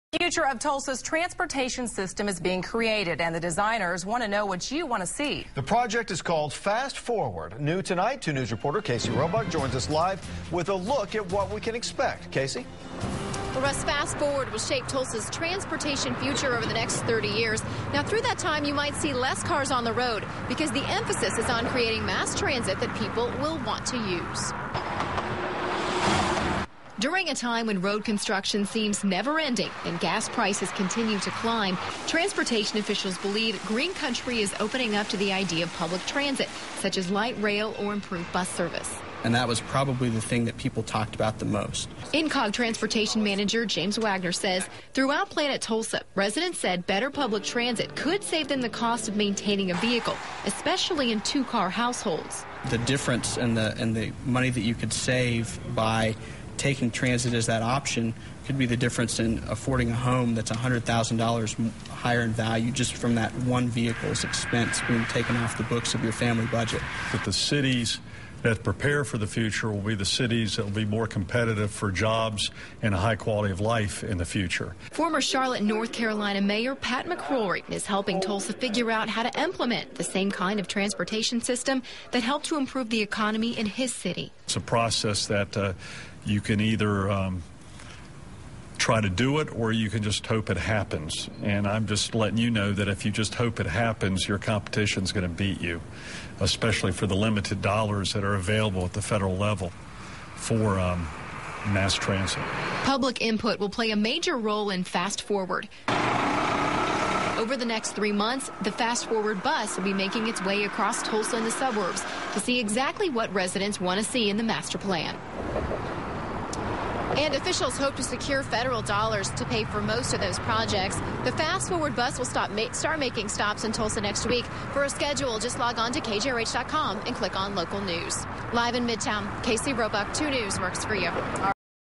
Tulsa Transit
Project Fast Forward will shape Tulsa's Transportation system for the next 30 years.